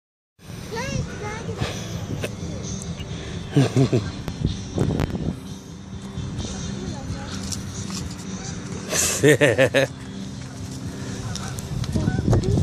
devin fishing
Devin Grimes Catching Rock Bass at Owasco Lake Emerson Park